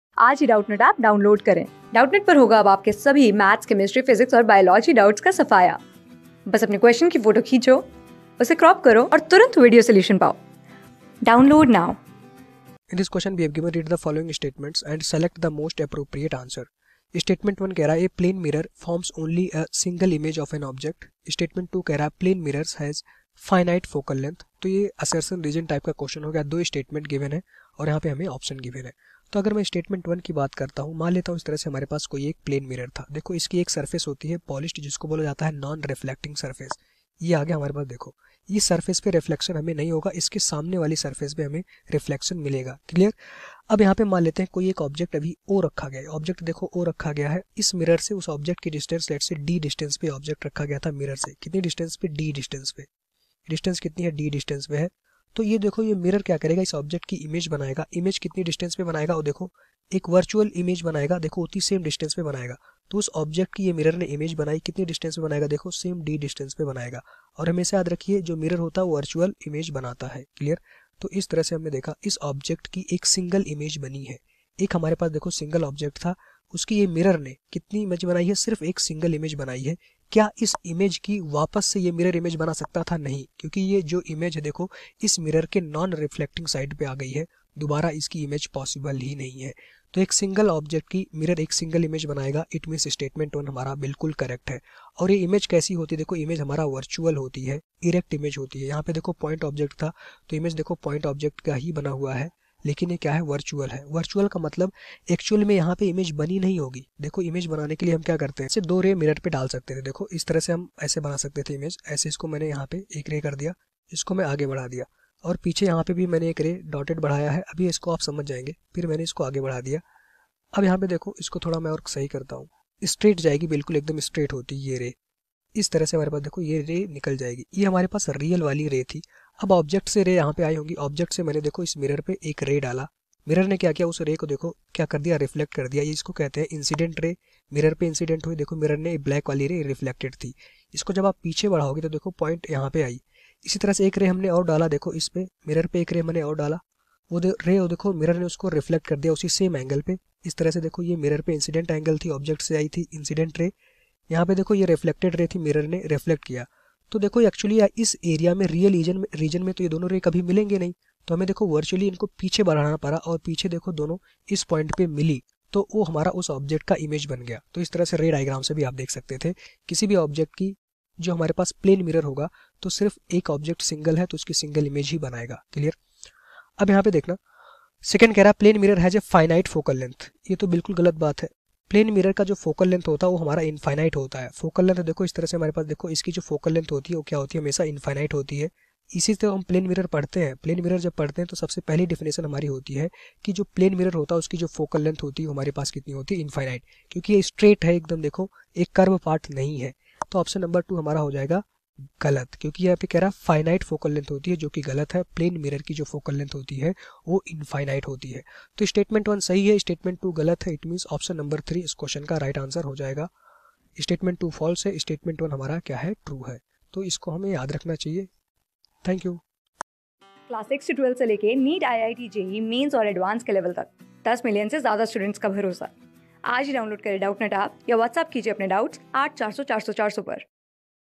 Read the following statements and select the most appropriate answer. Statement l : A plane mirr...
Read the following statements and select the most appropriate answer. Statement l : A plane mirror forms only a single image of an object Statement 2 : Plane mirror has finite focal length.
Class: 8
Subject: PHYSICS
Chapter: NSO MODEL TEST PAPER -1
Board:IIT JEE

👉 Have Any Query? Ask Us.